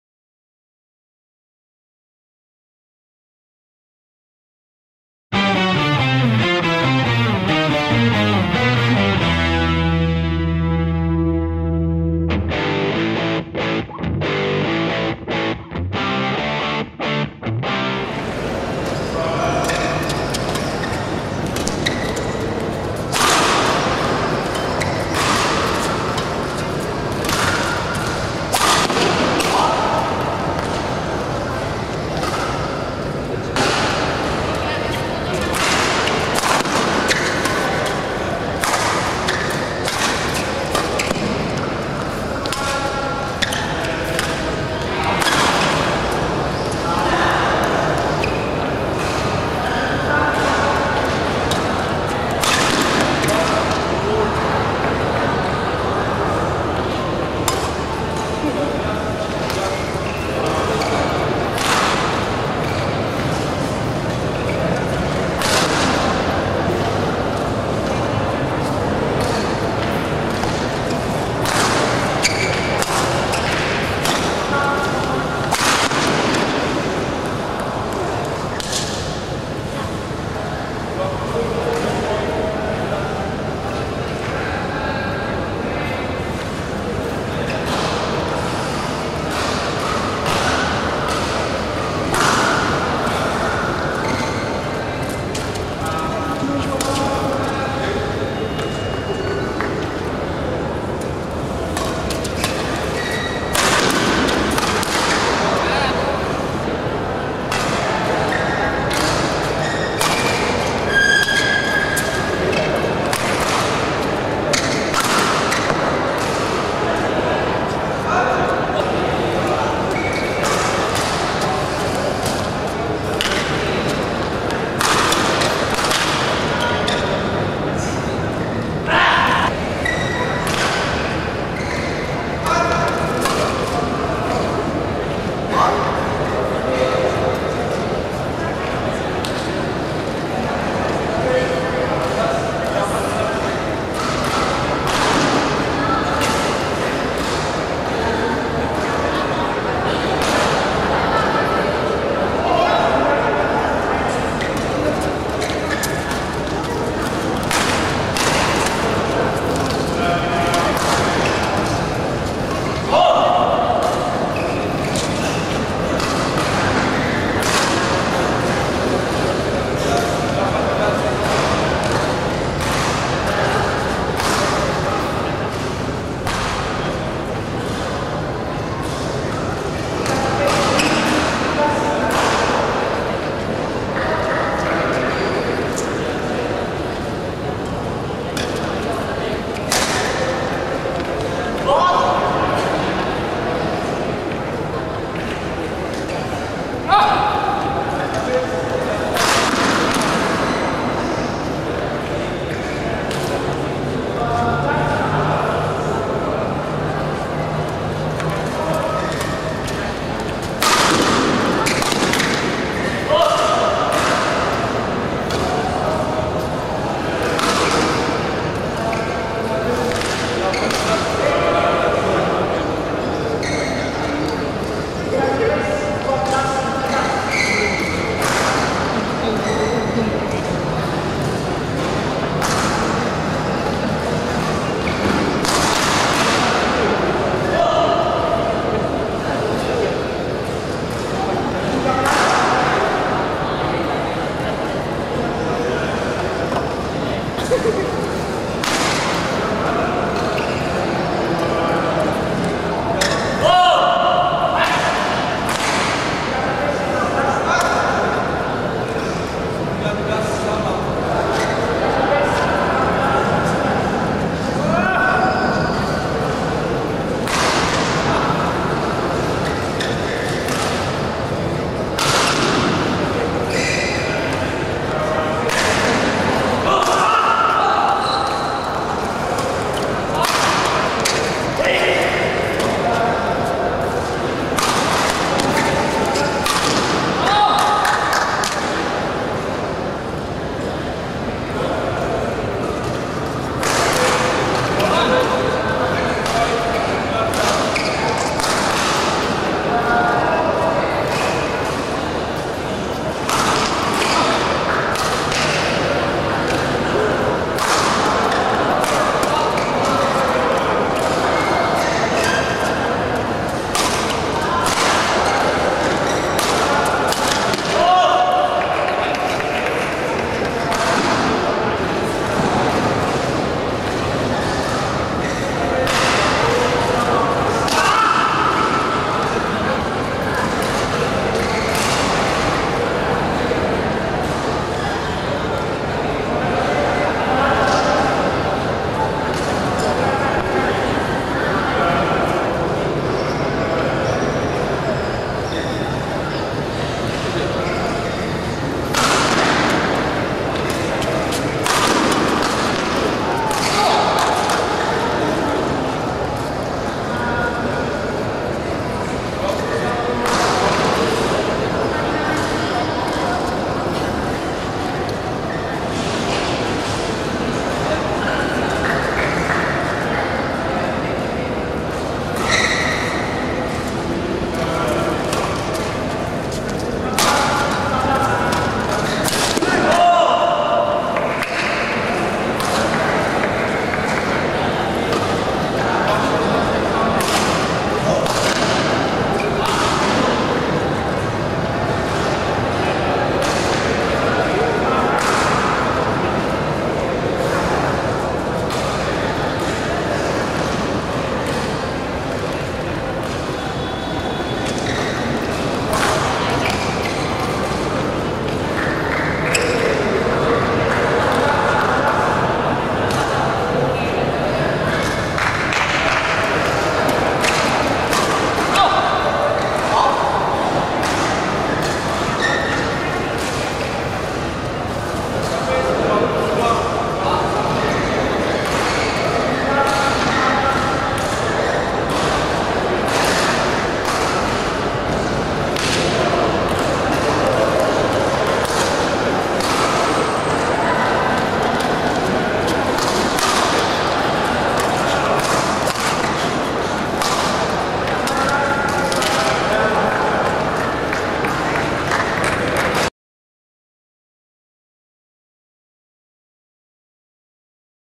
Gary Lam (HI QUA PRIMA) VS Indra Setiawan (PERTAMINA FASTRON)
Babak 1 Tunggal Dewasa Putera Djarum Sirkuit Nasional Jawa Timur Open 2014
03 - 08 November 2014 at GOR Sudirman, Surabaya - Jawa Timur